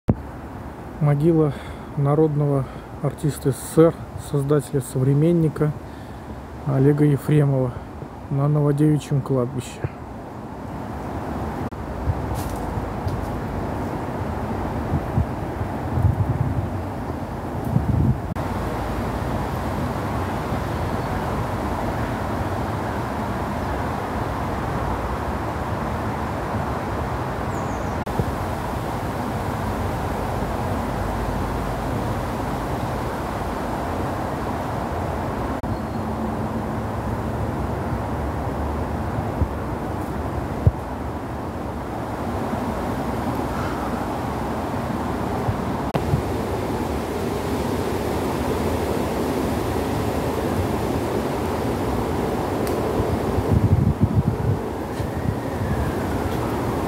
МОГИЛА НАРОДНОГО АРСТИСТА СССР ОЛЕГА ЕФРЕМОВА НА НОВОДЕВИЧЬЕМ КЛАДБИЩЕ МОСКВЫ
Оле́г Никола́евич Ефре́мов (1 октября 1927 — 24 мая 2000) — советский и российский актёр театра и кино, театральный режиссёр, кинорежиссёр, педагог. Герой Социалистического Труда.